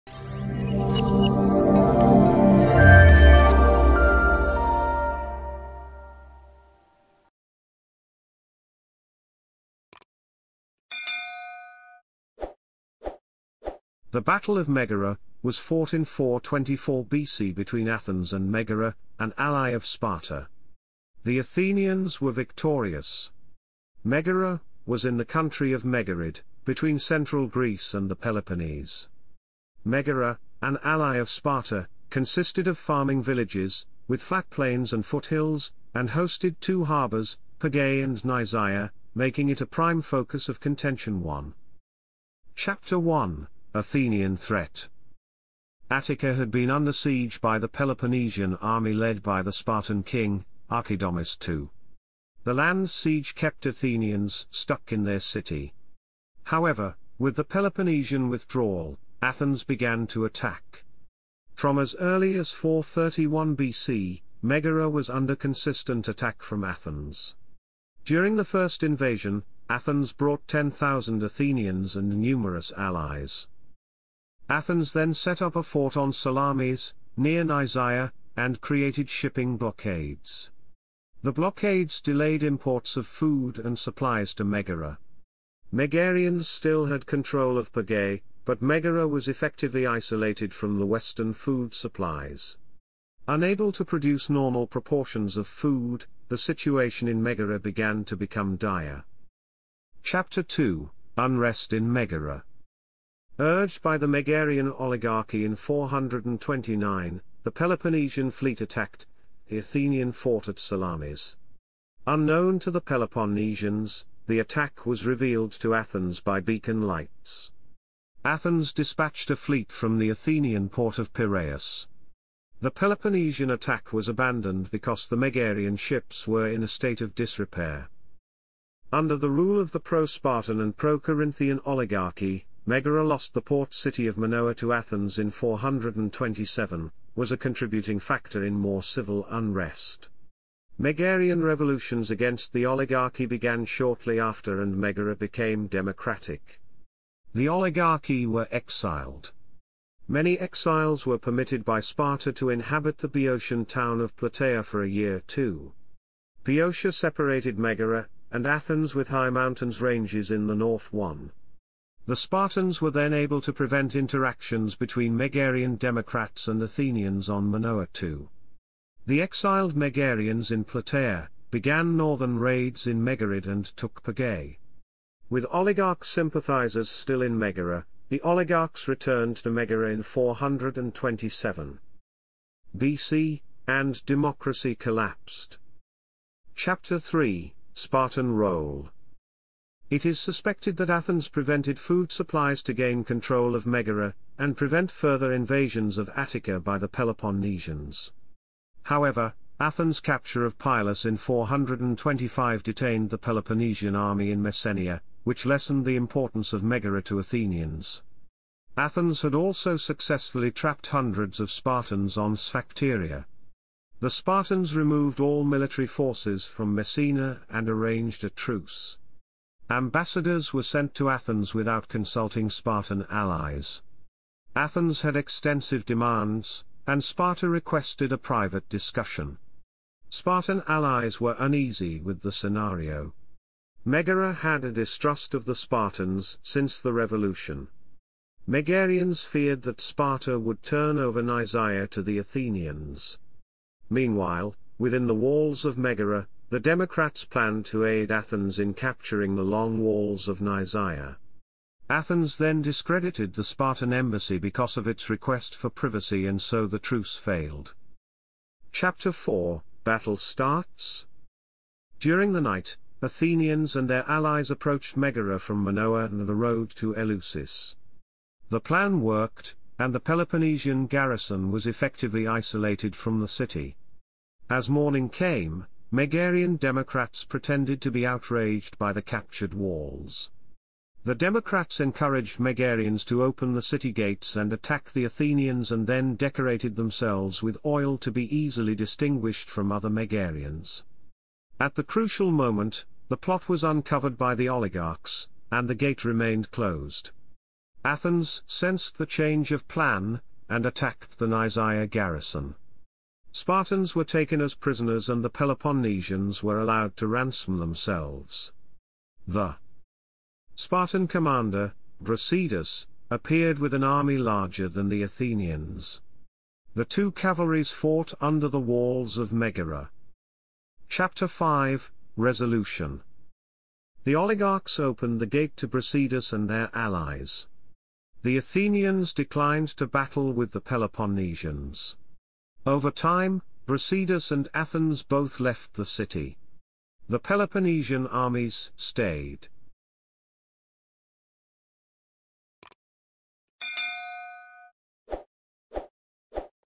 Battle of Megara - Wikipedia article
Video summary:
The Battle of Megara was fought in 424 BC between Athens and Megara, an ally of Sparta. The Athenians were victorious.